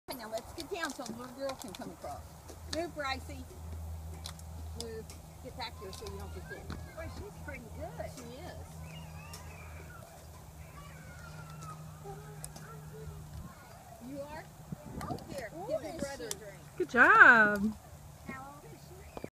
My little monkey
Samira is the queen of the monkey bars.  This was at Creekside playground. Age: 2 1/2.